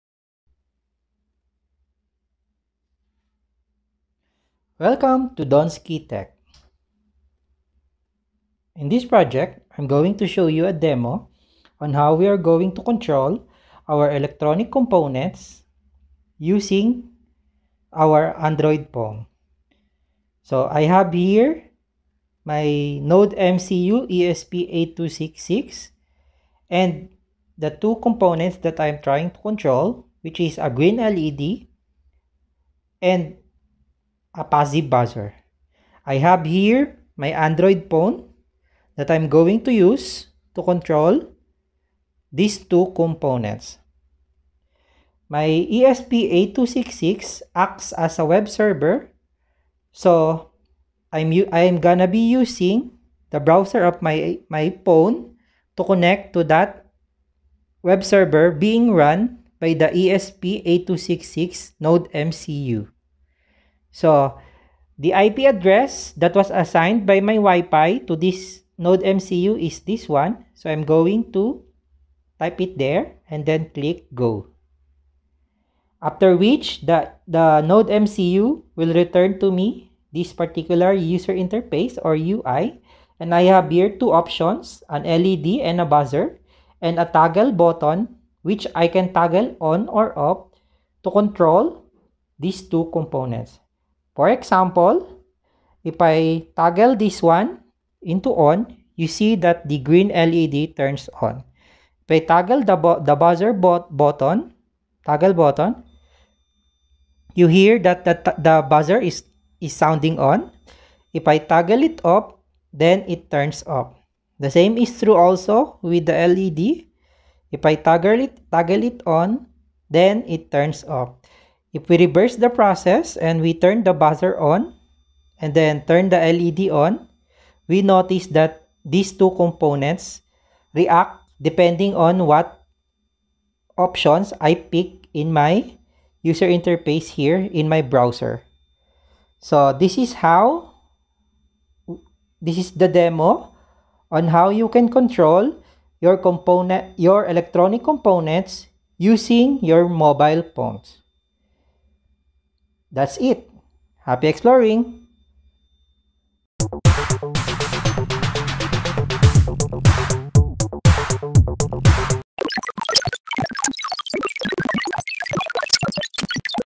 ESP8266 NodeMCU Web Server Using Arduino IDE: Control components from your Mobile Phones
In this video we are going to discuss how we can control our electronic components using our mobile phone thru the use of the ESP8266 Node MCU Web Server